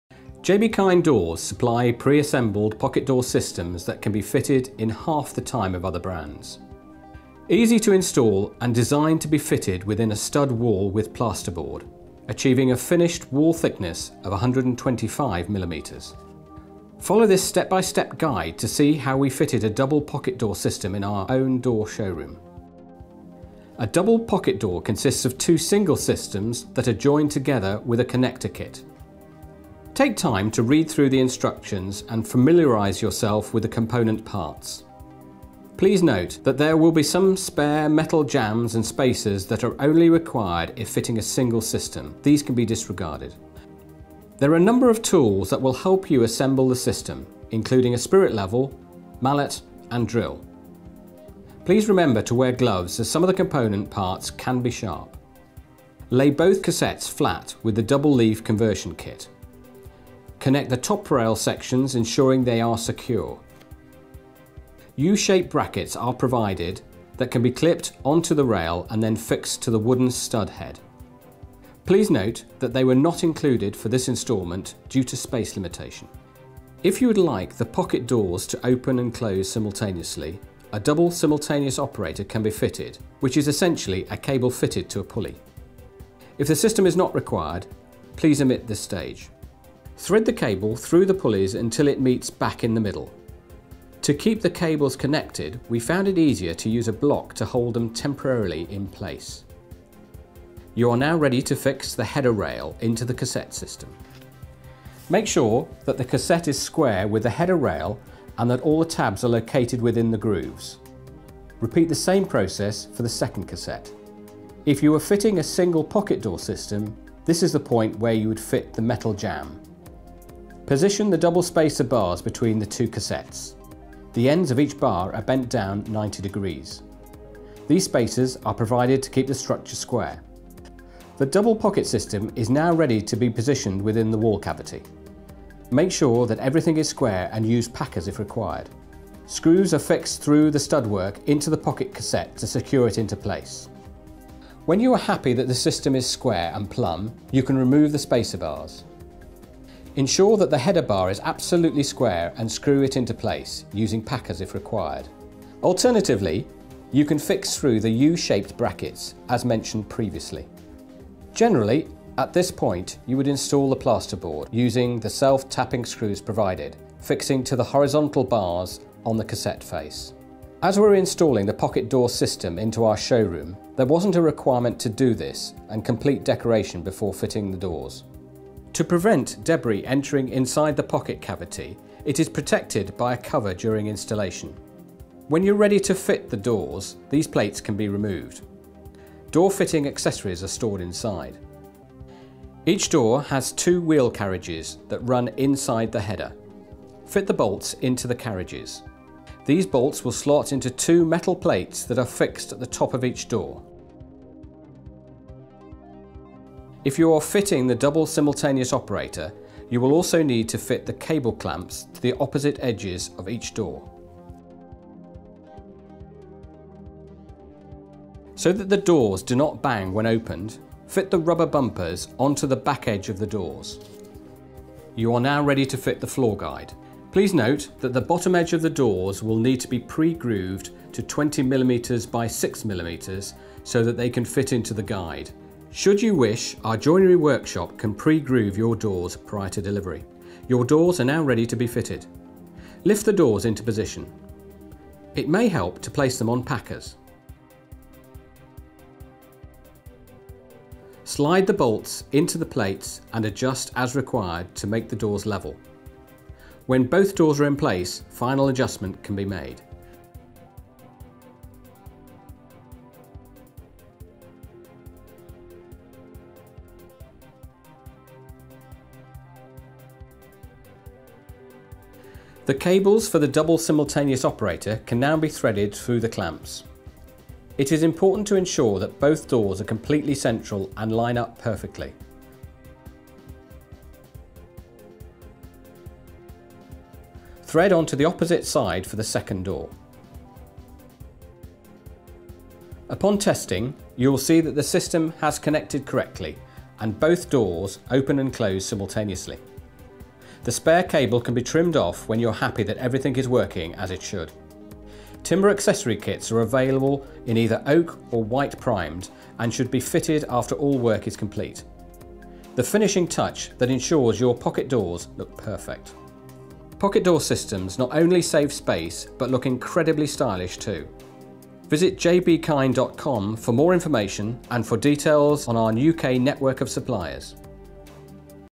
JB Kind How To Fit A Pocket Door System
Discover the simplicity of installing JB Kind's double pocket door system with our comprehensive step-by-step guide. Pre-assembled and effortless to set up, these systems seamlessly integrate within stud walls with plasterboard, offering convenience and efficiency in enhancing your living space with discreet and space-saving door solutions.